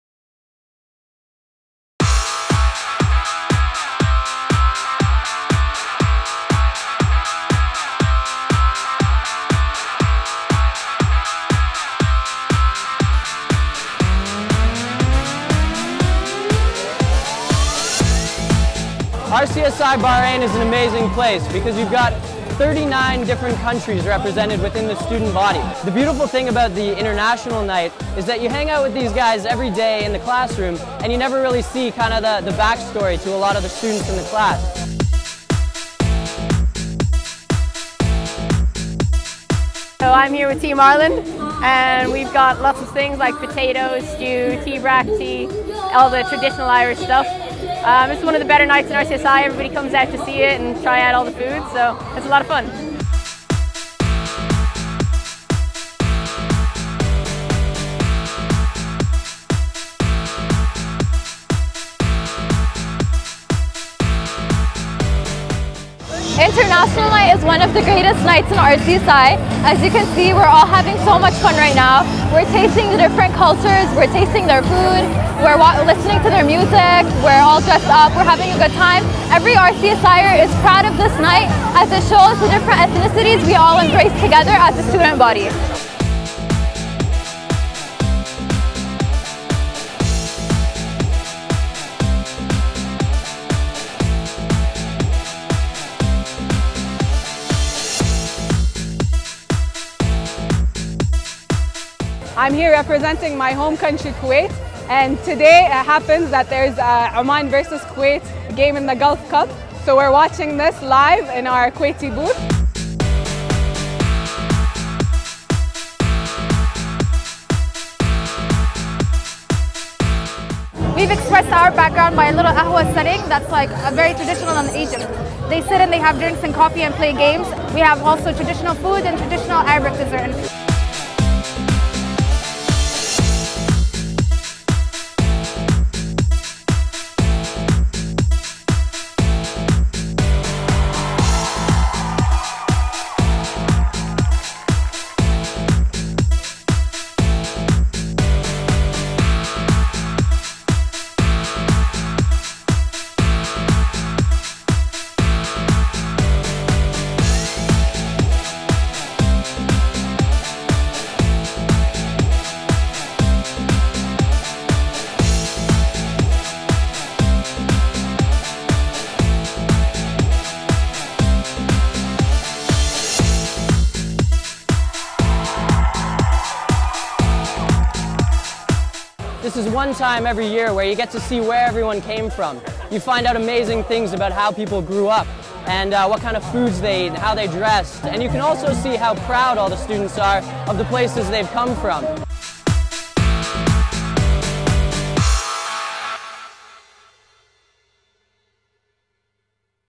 International Night
The annual International Night is one of the most popular events in the student calendar at RCSI Bahrain.
The Event brings a wide range of nationalities and backgrounds together for students to showcase some favourite traditions and cultures from their respective home countries.
The programme of events includes displays of traditional food, music, dance, fashion and sport from countries such as Ireland, Canada, Jordan, Kuwait, Lebanon, Sudan and Qatar. With more than 1,300 students from 39 countries, RCSI Bahrain offers courses in medicine, nursing and postgraduate studies.